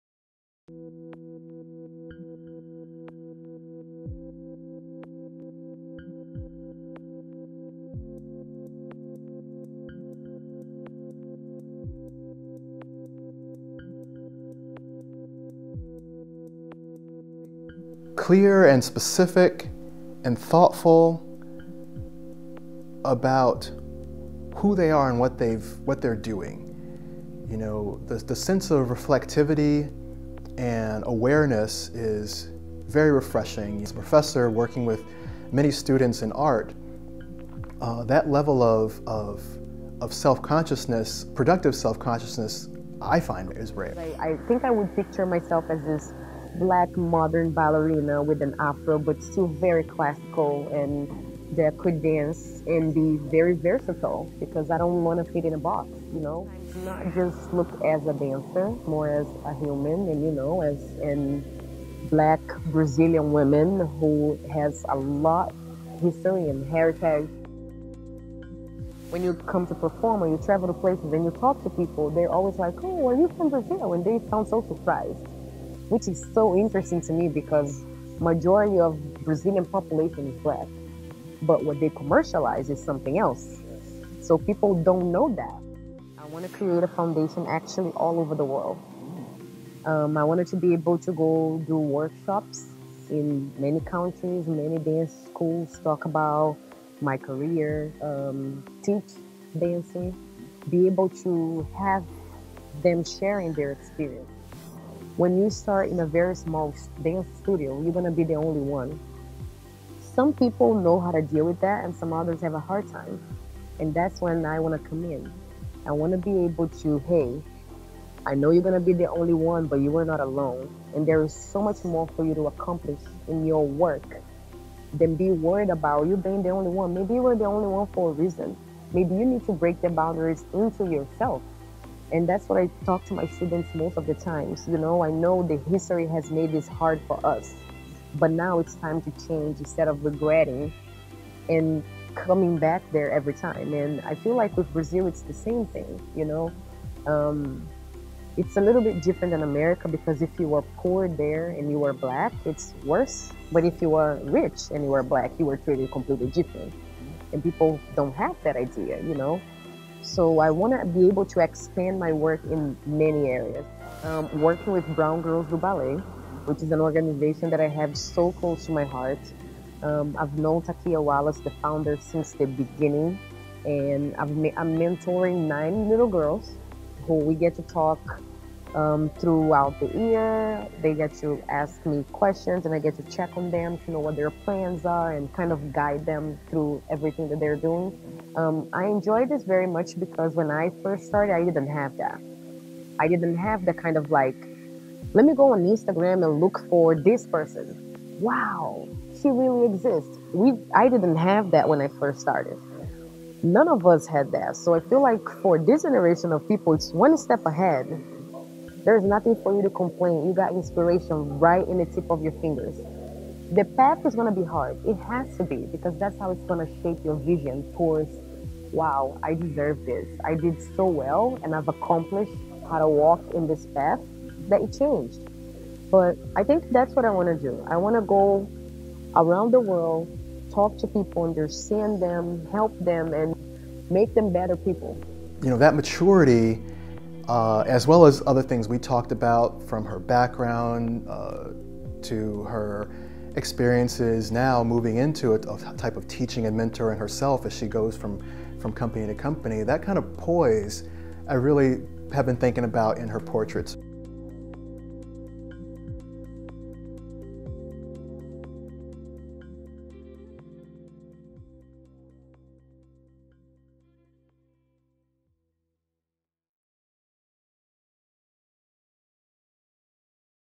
Counterpoint Project: Visual Artist Patrick Earl Hammie & Ingrid Silva
Talking Art:
Counterpoint Project explores, discusses, and reframes the ongoing cultural and critical contributions of black ballerinas in dance and visual culture. The title Counterpoint is a play on the ballet term pointe, which is a position on the extreme tips of the toe, and references an element that is juxtaposed and contrasted with another.